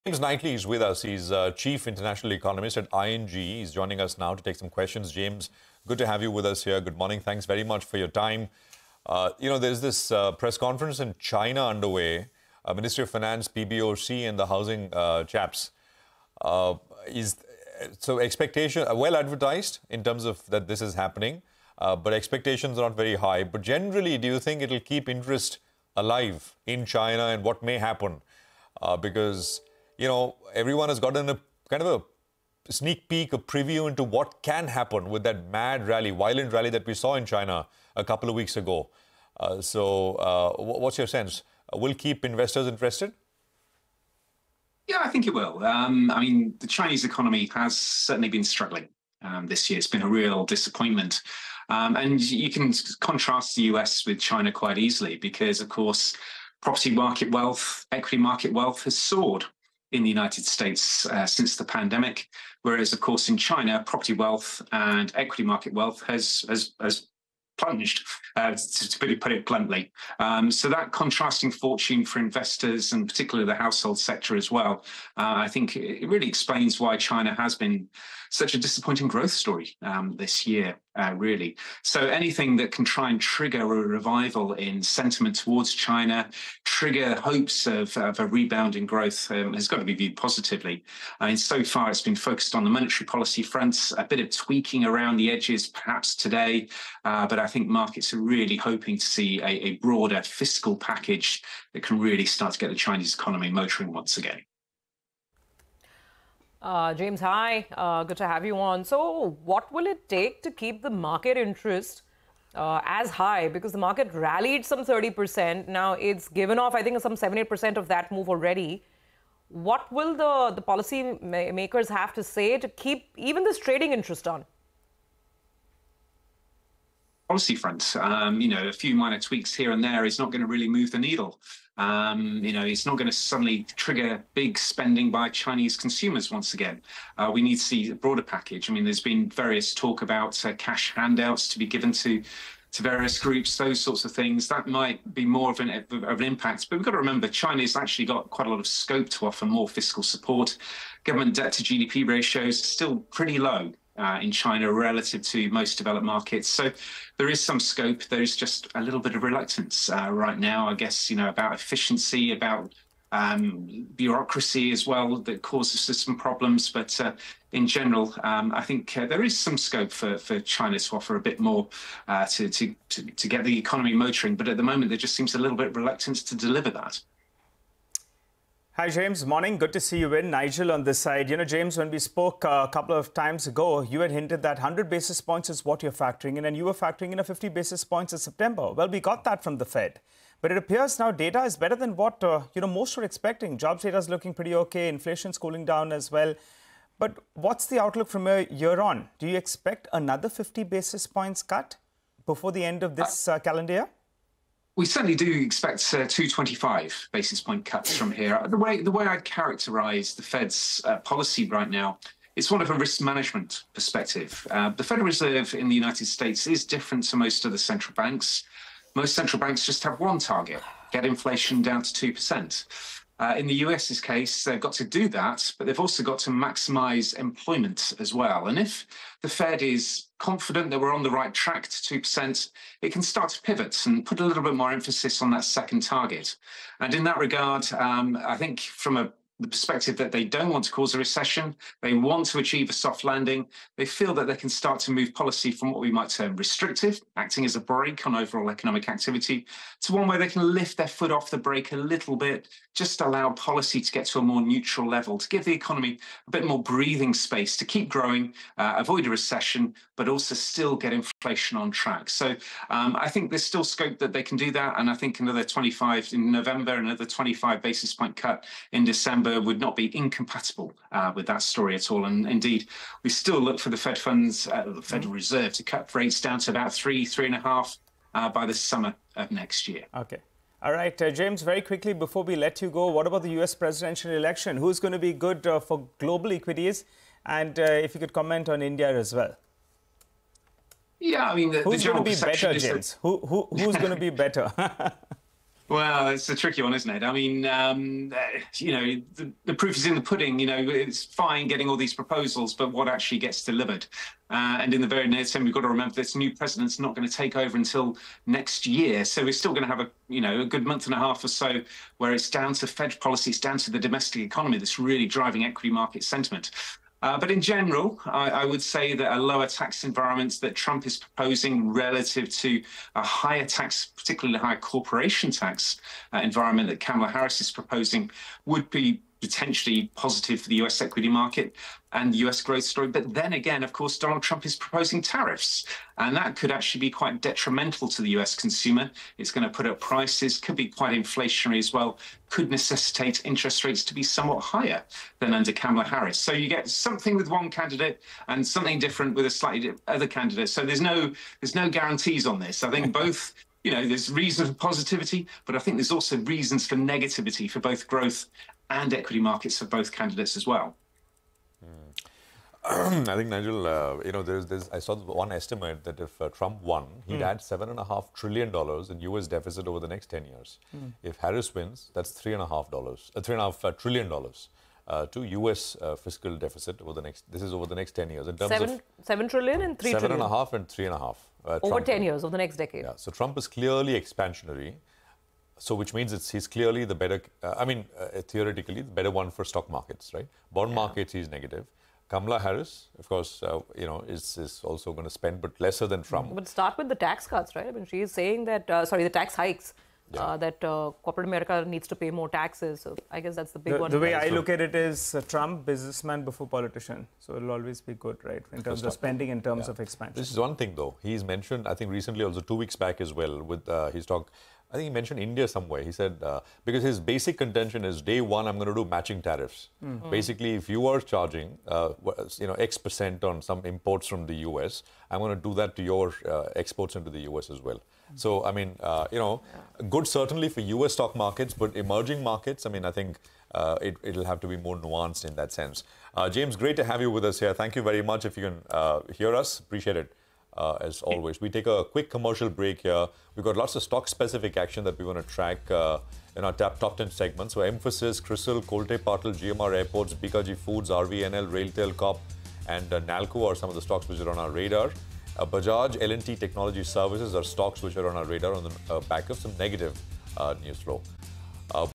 Chinese Eco Has Been Struggling; Markets Are Hoping To See A Broader Fiscal Package: ING | CNBC TV18
Global markets are hoping for a broader fiscal package from China, says James Knightley, Chief International Economist at ING. Tells Prashant Nair, Surabhi Upadhyay, & Nigel D'Souza that while there is room for additional measures to support the Chinese economy, there remains a reluctance to act.

n18oc_business